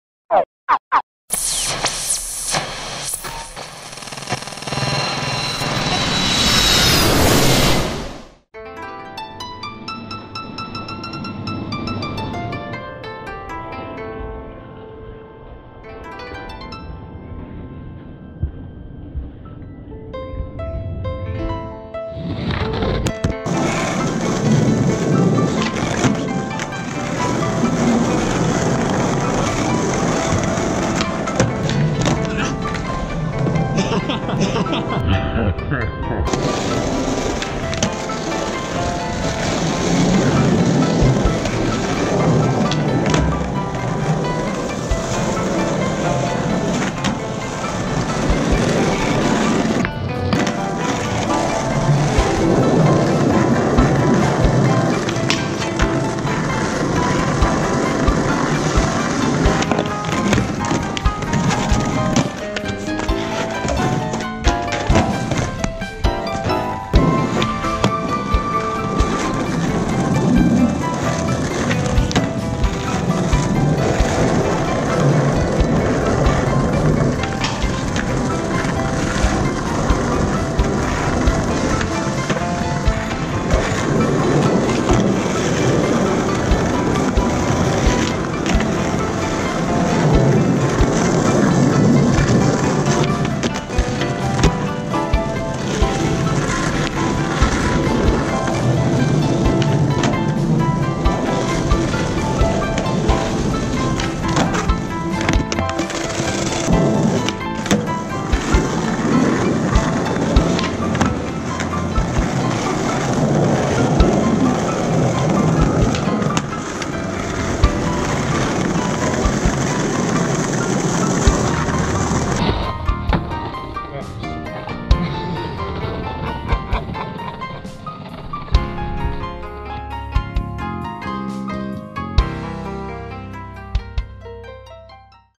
【2019】新年初滑り【スケボー】
今年もこんな自分をよろしくお願いします。
まだスノーボードしに行かないのか！？と言われそうですが年末年始混んでるので行ってないですww
そろそろ滑りに行く予定でございます！